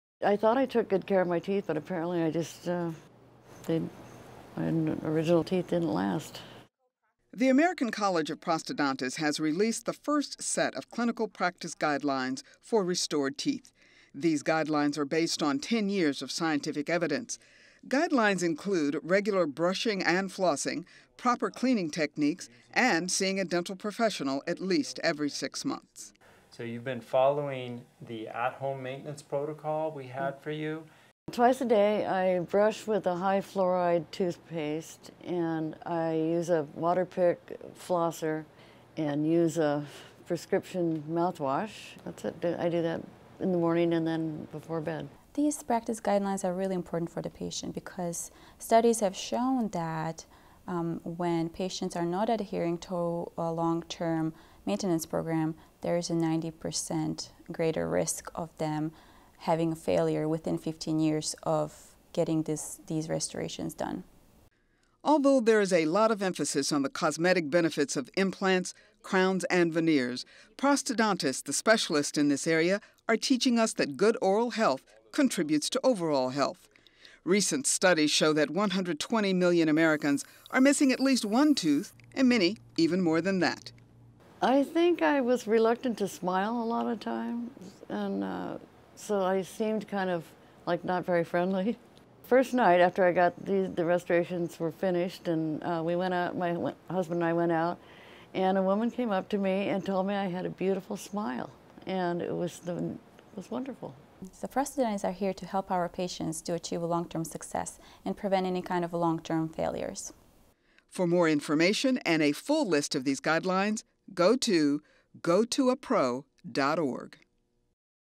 Colorado Springs Prosthodontist Dr. Marina Funtik - Guidelines Minimize Risk of Failure by 90%
ACP and Dr. Marina Funtik discuss the new clinical practice guidelines for restored teeth and how following them can minimize risk of restoration failure by 90%.

Visit GoToAPro.org to find a prosthodontist near you.